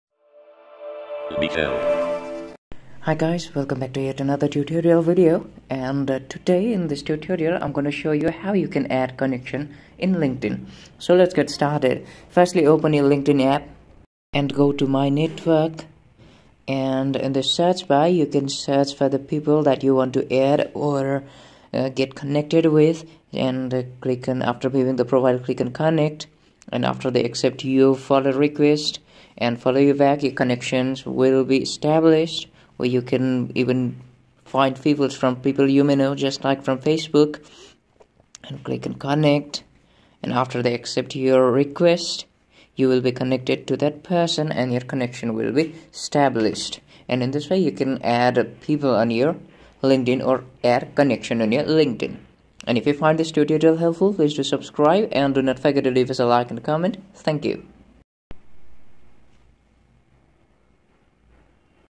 How to Add more Connections on LinkedIn
LinkedIn is a professional-oriented social network used by millions of professionals to connect and share career information, find jobs, and create business opportunities. It also allows users to create profiles, post resumes and network with other professionals. LinkedIn provides opportunities for job seekers to showcase their skills and qualifications, learn about new job openings, make professional connections, and find mentors. LinkedIn has become an invaluable tool for today's businesses.
In this video we will learn how to Add more Connections on LinkedIn. Follow us and enlarge your connection on LinkedIn account.

00:00 How to Add more Connections on LinkedIn
00:15 Manage Network
00:35 Connect people